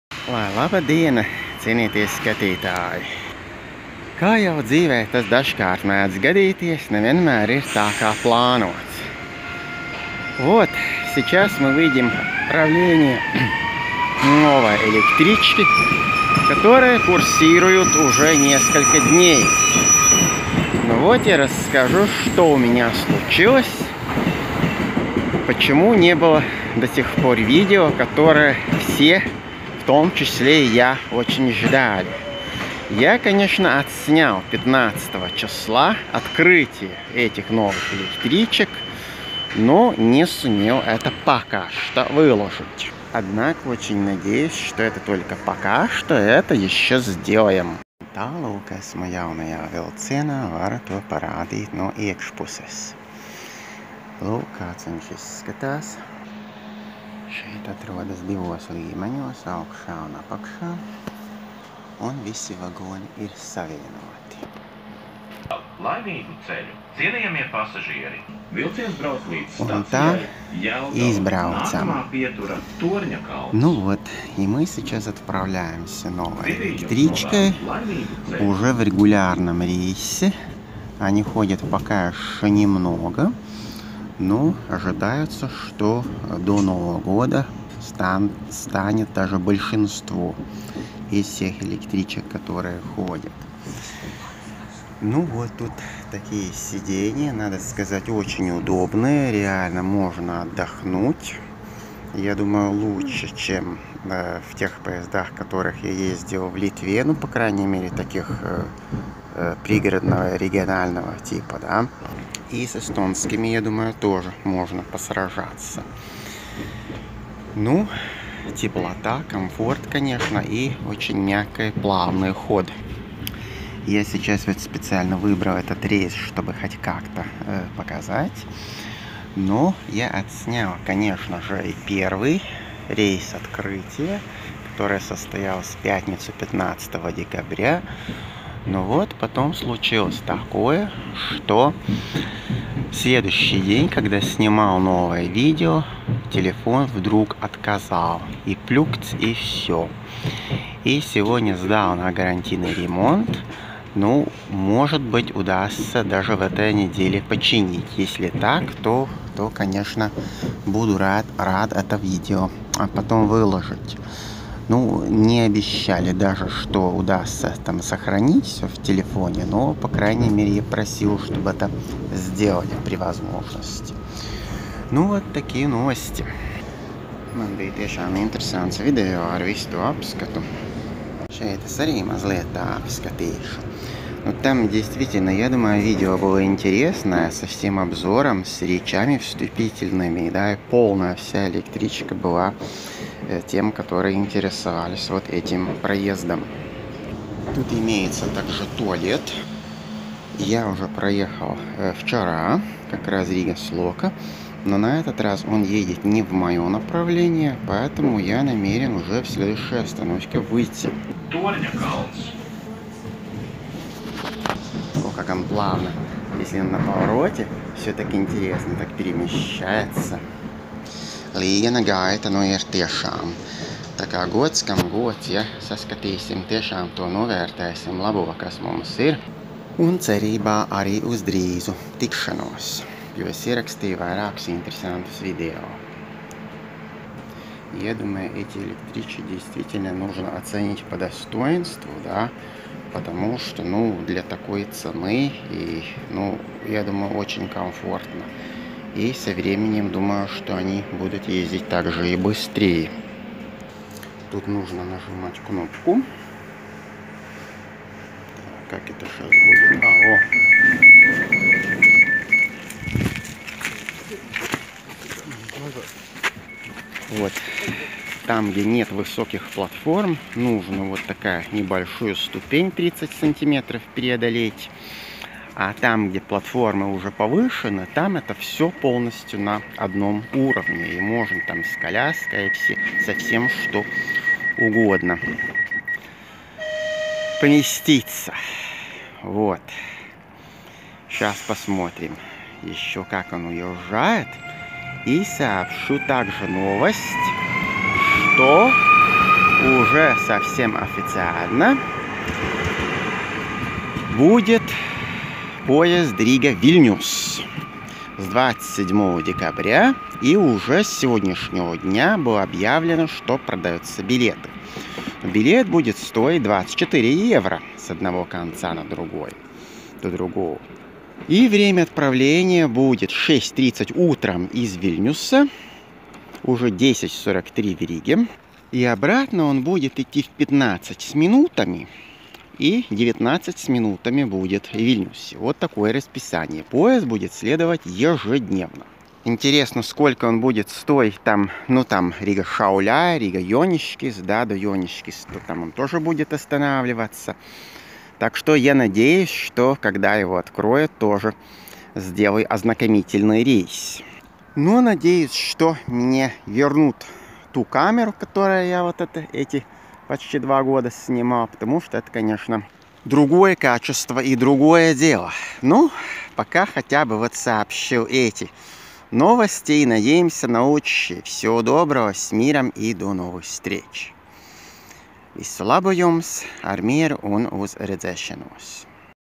Новые поезда уже ездят в Латвии! |Что случилось? | Анонс Рига-Вильнюс!
С 15 декабря в Латвии стали ездить новые электропоезда Škoda 16EV. Я сделал видео на мероприятии открытия и первого рейса. Но, к сожаленю, пока его выложить не удалось. Почему? Расскажу в этом видео.
Также покажу короткую поездку новой электричкой и сделаю анонс другого события что нас ожидает уже в ближайшее время: открытие международного сообщения между Вильнюсом и Ригой!

Jaunie Škoda elektrovilcieni jau darbībā!
Pastāstīšu par atklāšanas dienas video, izbrauksim nelielu gabalu jaunajā vilcienā un iepazīstināšu ar aktuālo par starptautisko satiksmi Rīga-Viļņa!
Patīkamu skatīšanos!

The new EMU's Skods 16EV in operation in Latvia! First days and first trains.
Also train route Riga - Vilnius will be opened soon!